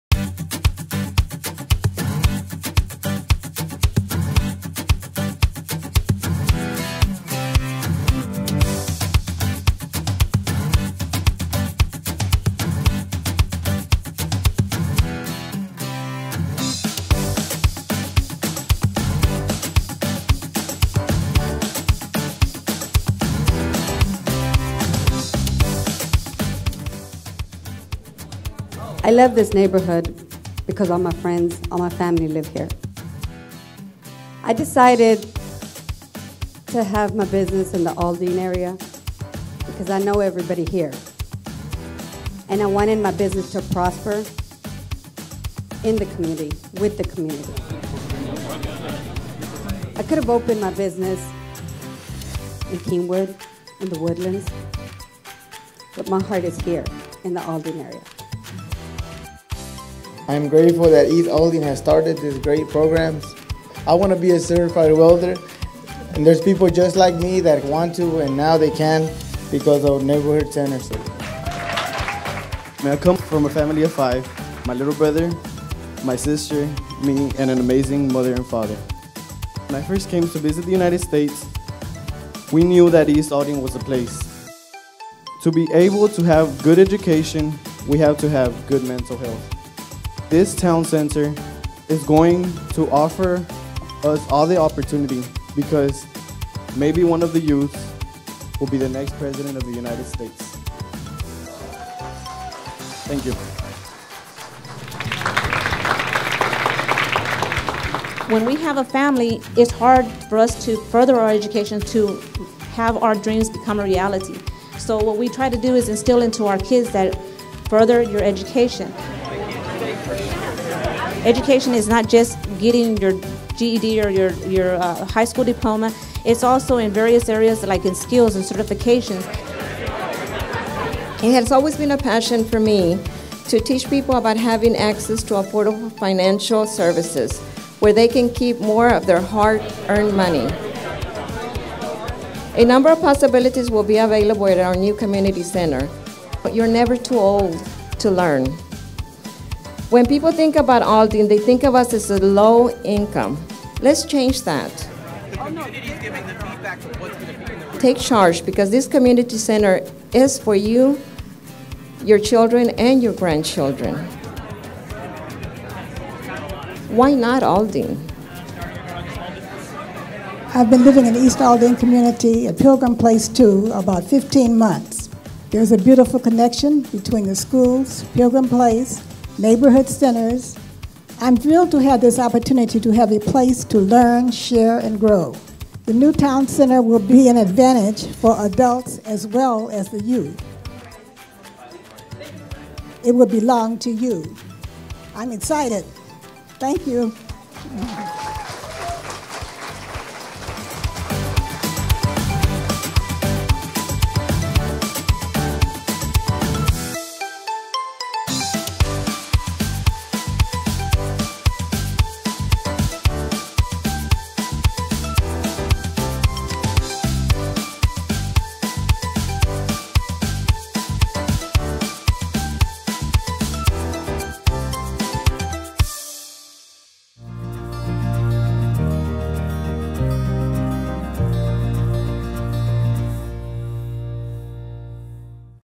East Aldine Building Upon What Works
Residents imagine a community where they can continue their education and learn new skills; where a pathway to better-paying jobs exists; where small businesses are thriving; where a community center provides opportunity and connection.

Residents imagine their lives in East Aldine.